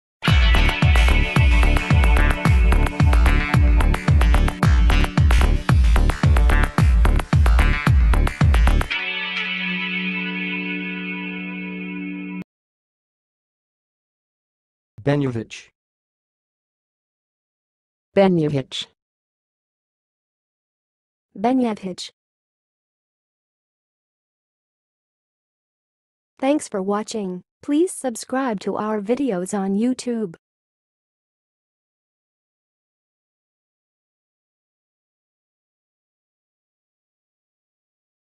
BENYAHICH - HOW TO SAY BENYAHICH?
BENYAHICH - Say Benyahich like an expert: Pronunciation guide in 2023

Benyahich is a family name of North African origin, commonly found in countries like Algeria and Morocco.
It is derived from the Berber language and is often considered a tribal or regional marker.
The name Benyahich is often associated with strong family ties, hospitality, and a rich cultural heritage.

Pronunciation:
To pronounce Benyahich, you can break it down into syllables: Ben-yah-ich.
The 'Ben' is pronounced like 'ben' in 'bench', the 'yah' is pronounced like 'yah' in 'yacht', and the 'ich' is pronounced like the 'ich' in 'rich'.
Another variant of the pronunciation is ben-ya-heesh.

Test yourself by speaking the following examples:
1. Benyahich is a common surname in Algeria
2. Many people with the last name Benyahich trace their roots back to Berber tribes
3. The Benyahich family is known for their strong sense of community and hospitality
4. I met someone named Benyahich at a cultural event showcasing Moroccan heritage
5. The name Benyahich represents the rich cultural diversity of North Africa